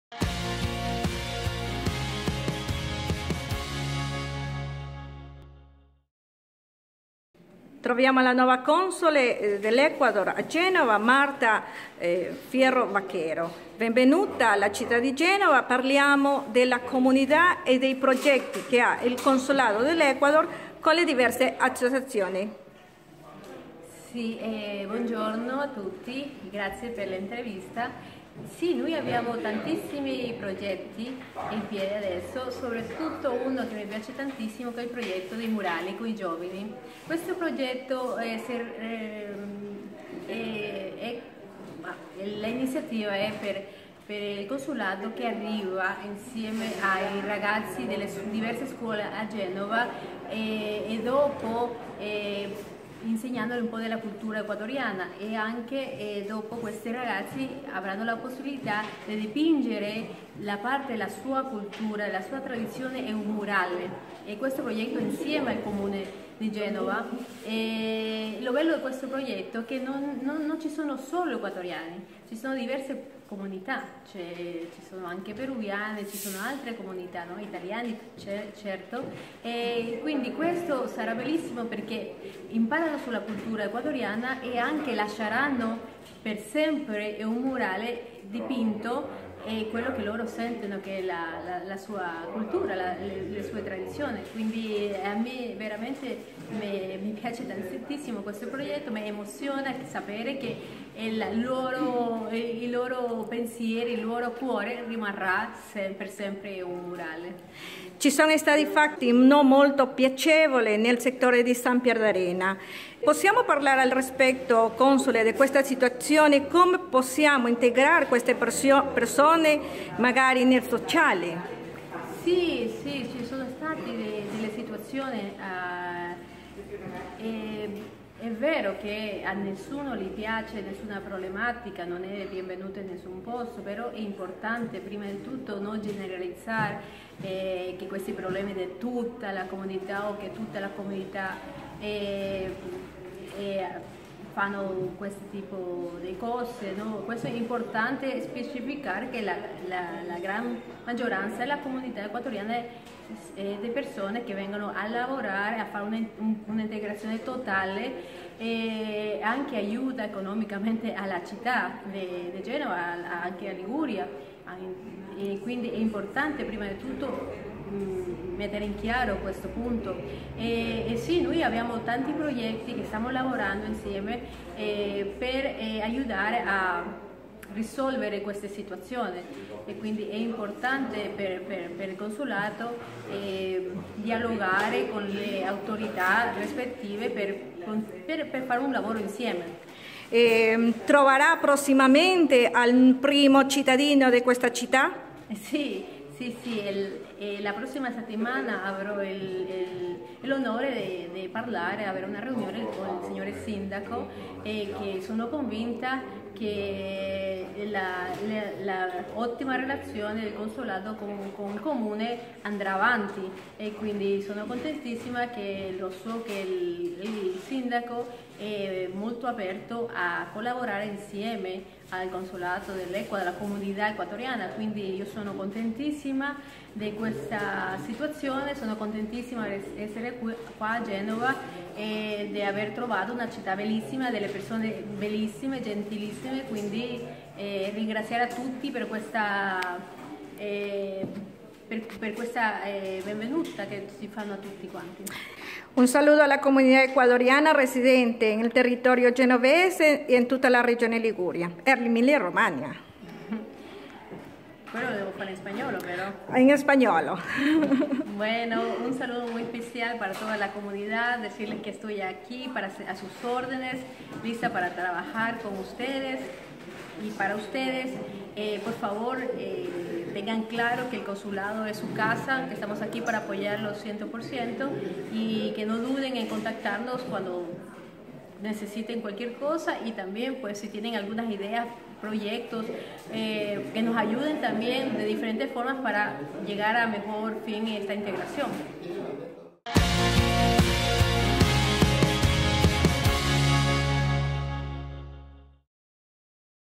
La Gran Maestra de Ajedrez Martha Fierro Baquero, es la nueva Cónsul del Ecuador a Génova
La Gran Maestra Internacional de Ajedrez Martha Lorena Fierro Baquero, es la nueva Cónsul del Ecuador a Génova, joven ecuatoriana que marcó una era en el escenario deportivo ecuatoriano y de América Latina, adjudicándose importantes podios en las arenas internacionales. Ha representado el Ecuador en varias Olimpiadas de Ajedrez, Panamericanas, Suramericanos, Bolivarianos, abiertos internacionales, entre otros. En la actualidad Martha Fierro, es la deportista  ecuatoriana que posee el cargo más alto en una organización deportiva a nivel mundial, en este caso la de vicepresidenta de la FIDE (Federación Internacional de Ajedrez) y Vice Presidenta de la Academia Internacional de Ajedrez fundada a Perugia